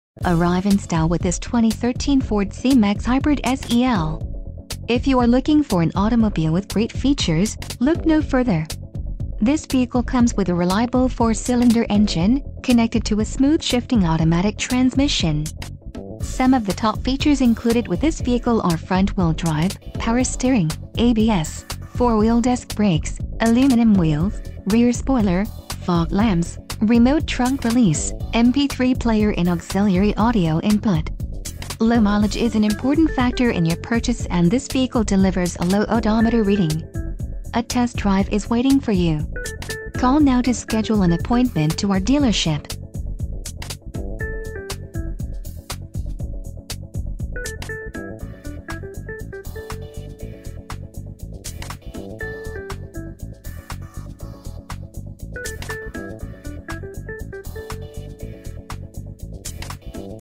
2013 Ford C-Max Hybrid SEL - AutoMatch - Chicago - Morton...
Panoramic Roof, **BACKUP CAMERA**, **BLUETOOTH, HANDS FREE**, **CLEAN CARFAX**, **HEATED SEATS**, **LEATHER**, **ONE OWNER**, **STILL UNDER FACTORY WARRANTY**, and 2013 Ford C-Max Hybrid SEL in Ice Storm Metallic vehicle highlights include. Equipment Group 303A, Hands-Free Technology Package (Power Liftgate and Rear-View Camera), Parking Technology Package, and Premium Audio & Navigation Package. If you've been hunting for the perfect 2013 Ford C-Max Hybrid, well stop your search right here. This is the family car that fits the needs of you and your kids. Wishes really do come true.